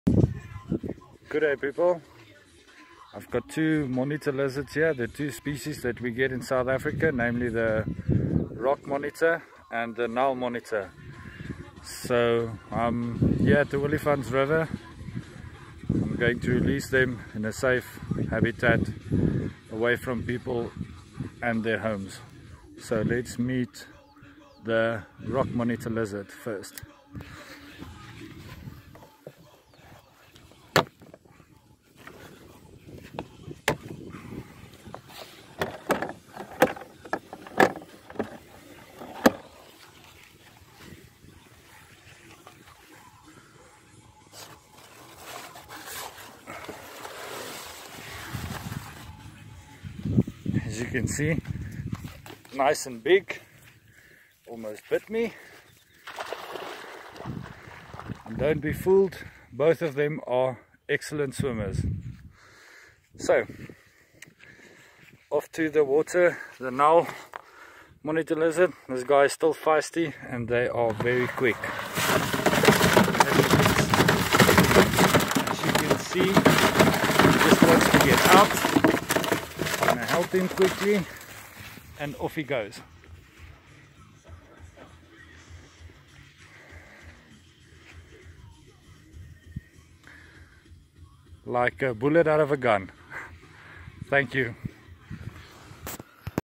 Relocation of monitors
Had 2 monitor lizzard rescues the past few days the rock monitor was caught in a office and the nile monitor was caught in a vehicle engine bay. Both were safely relocated at the olifants river..